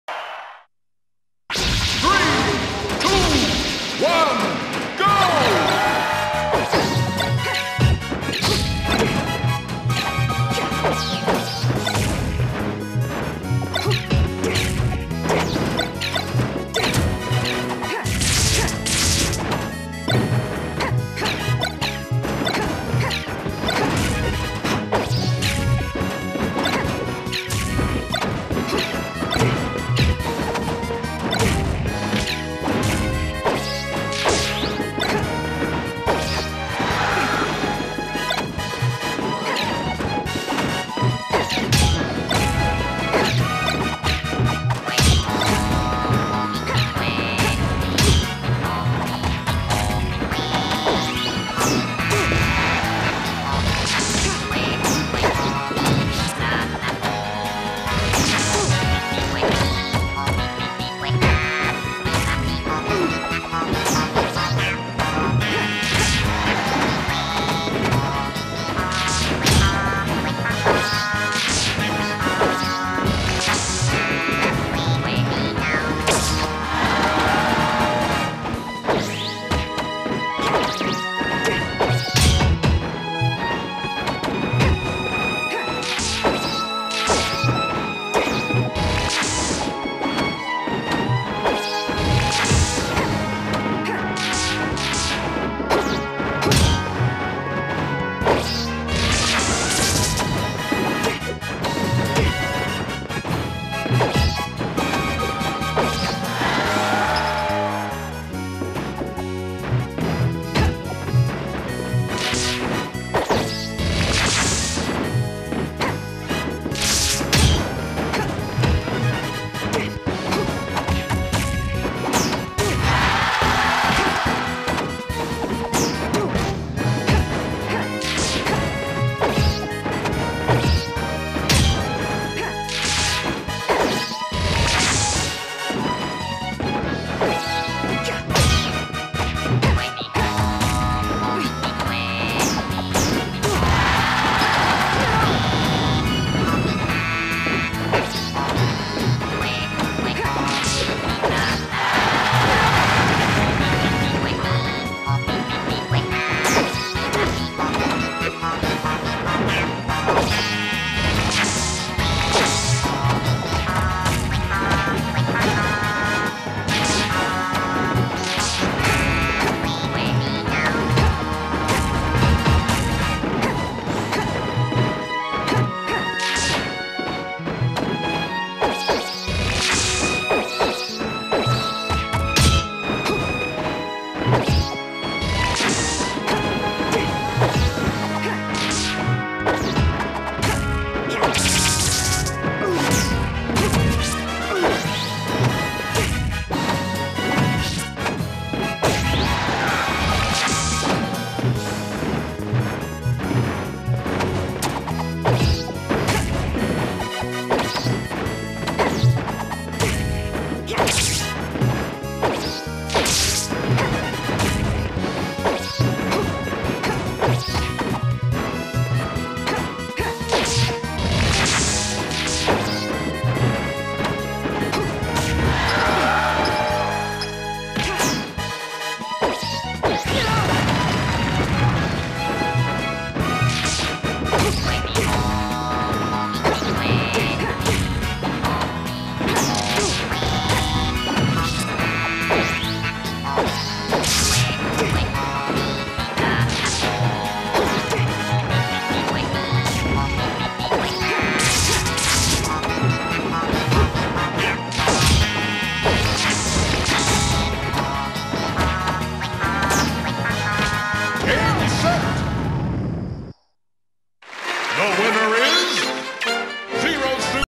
Crew Battle 14/15 : Charby (Blue ZSS) vs SSJ (Purple ZSS)
Crew Battle : Baaka Team vs Sans Papiers (14/15).
Charby (Blue ZSS) vs SSJ (Purple ZSS)